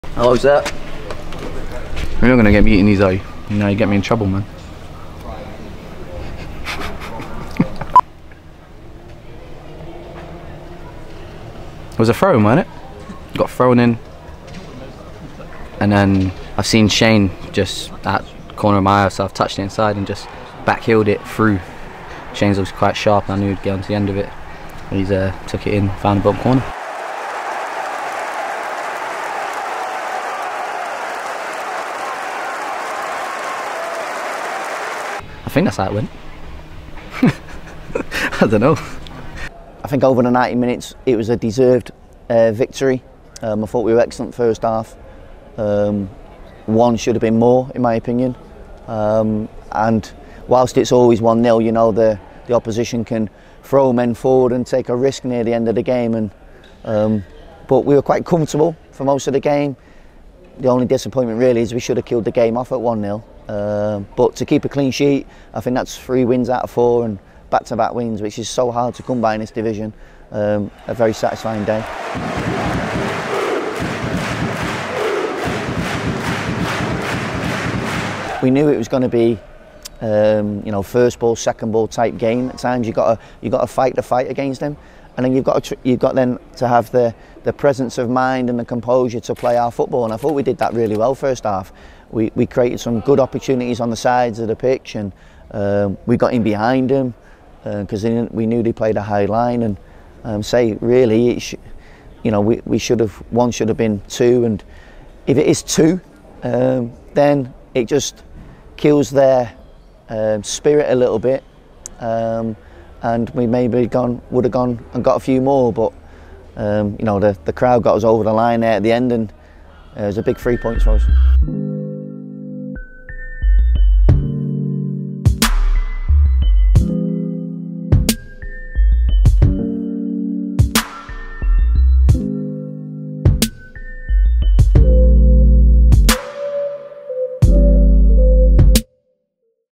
Barnsley Reactions | Keshi & Critchley
Keshi Anderson and Neil Critchley share their thoughts after Blackpool's 1-0 victory over Barnsley in the Sky Bet Championship.

► Sign up to Tangerine TV for access to live streams and more behind the scenes content!